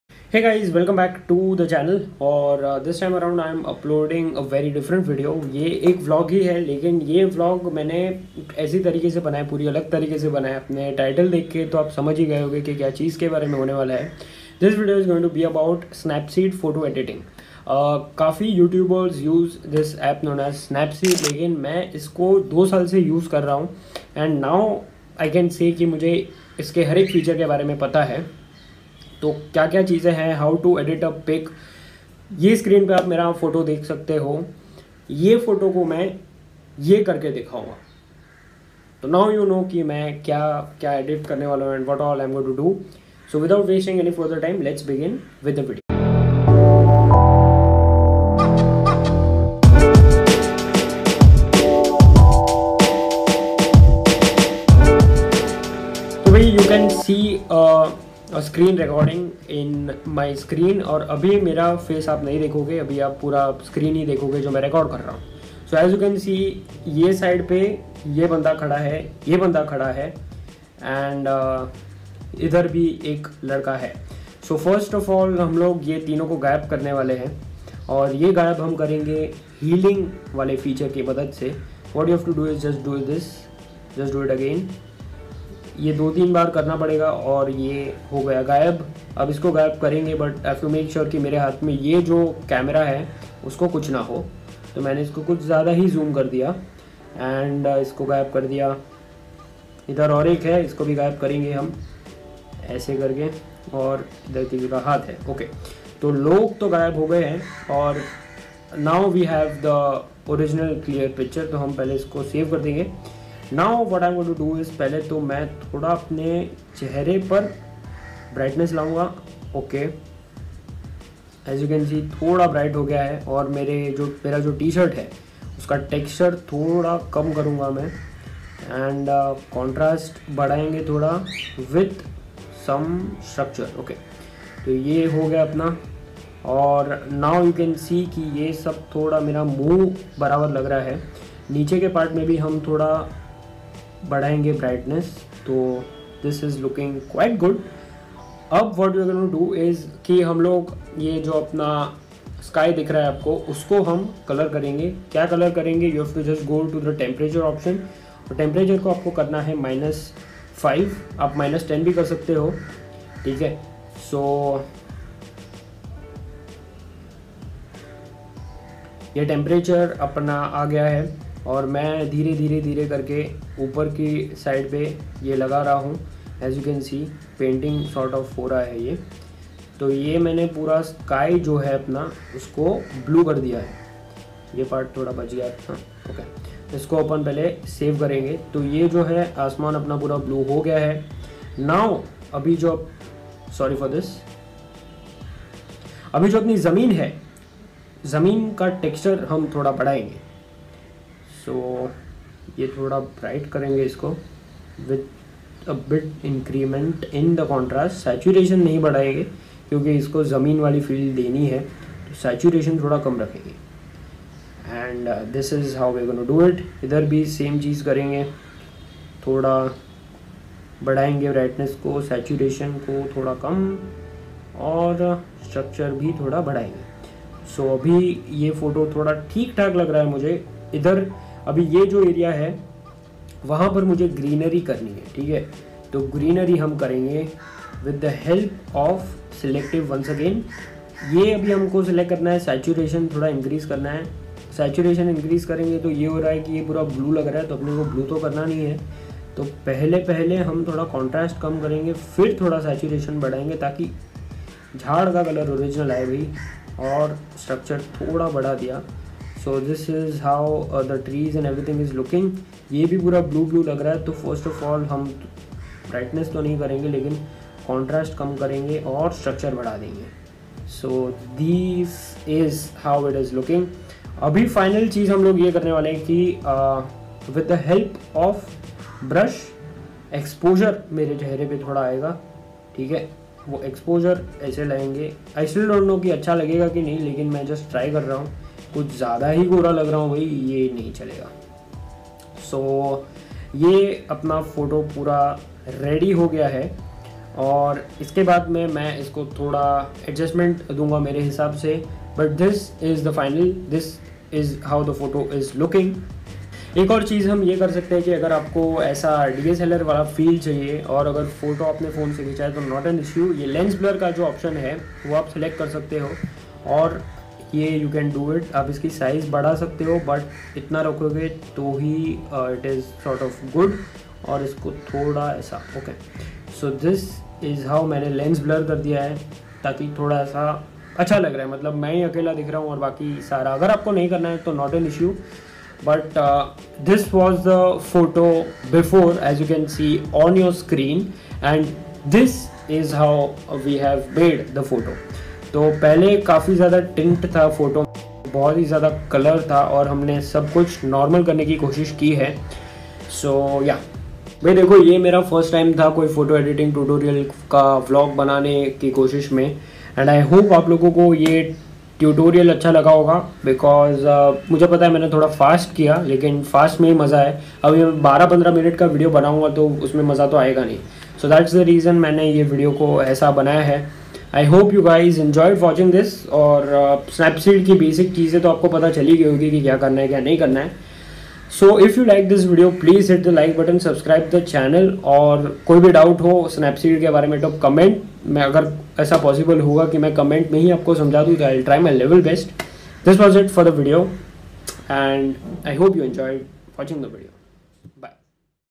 HOW to EDIT your PHOTOS on SNAPSEED🖼️🔥|Snapseed Photo Editing
Hey guys. Trying something veeeery different from my usual vlogs. In this one I have tried to teach you guys how to do basic photo editing using the Snapseed app. This was just a Level 1 editing. I know much more about this but first, let us just learn the basics.